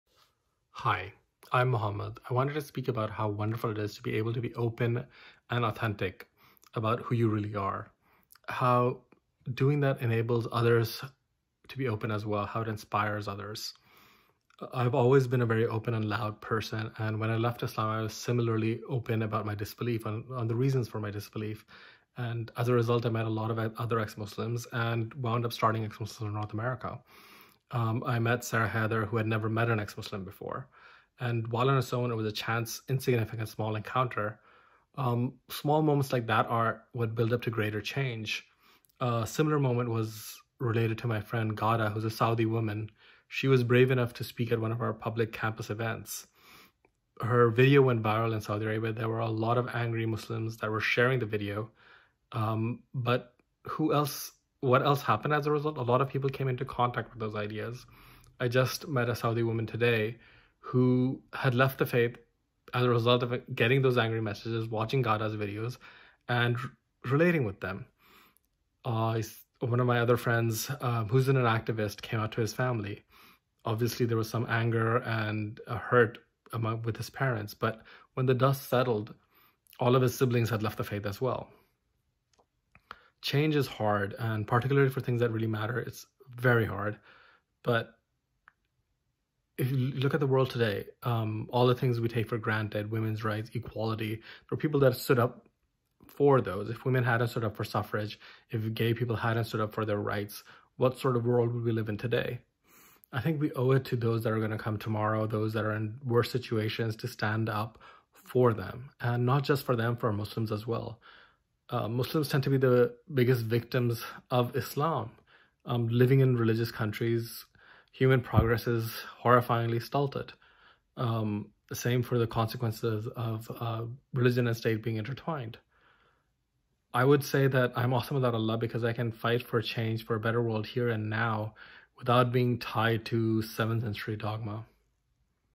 Muhammad Syed - "Change is hard, particularly for things that really matter" AwesomeWithoutAllah
Muhammad's very open personality as an ExMuslim ultimately led to the founding of EXMNA. For many, meeting an ex-Muslim forms a key part of their journey out of Islam. By speaking up, you help others realize they too can be